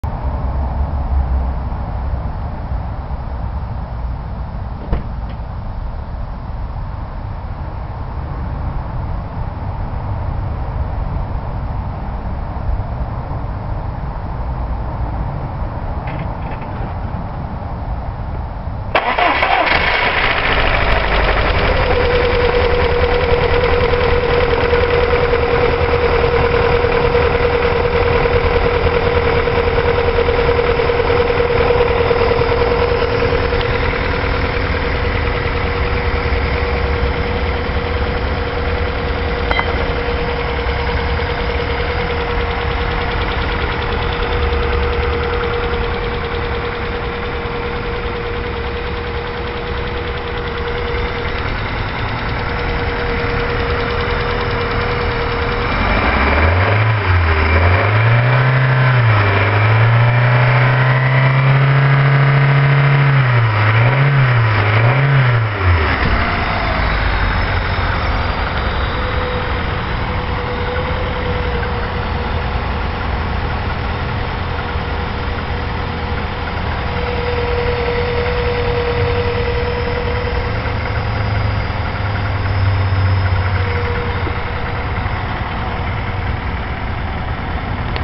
Rover 45 start up after head gasket change
intitial start up after replacing the head gasket, wobbly engine as the top mount wasnt fitted at that point